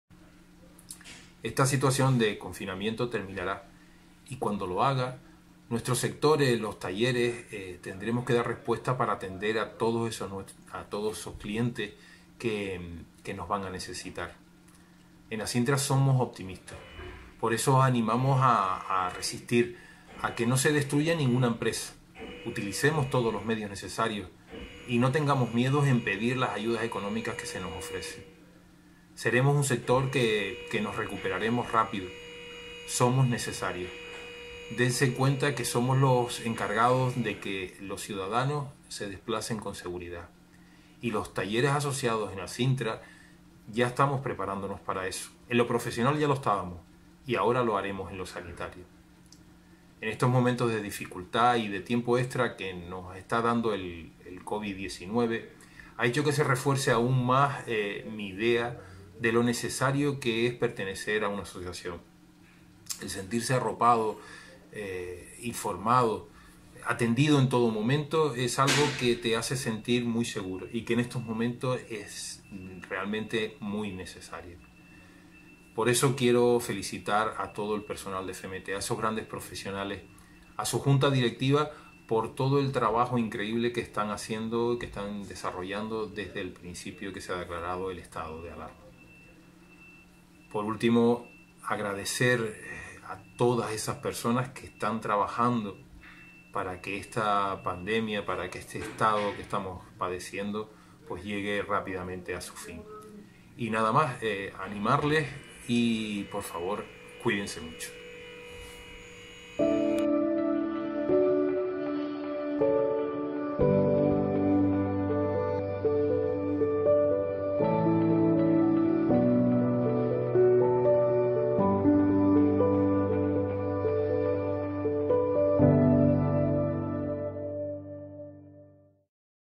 Femete ayuda: mensaje del secretario de Asintra
Femete ayuda es una nueva iniciativa que promueve la federación para difundir mensajes positivos y motivadores. En este caso, Francisco Afonso, propietario del Taller Vica Motor y secretario de Asintra (Asociación Industrial de Talleres de Reparación de Automóviles de la provincia tinerfeña) y vicepresidente de Femete, nos envía ese vídeo dirigido a empresas, profesionales, trabajadores/as y sus familias.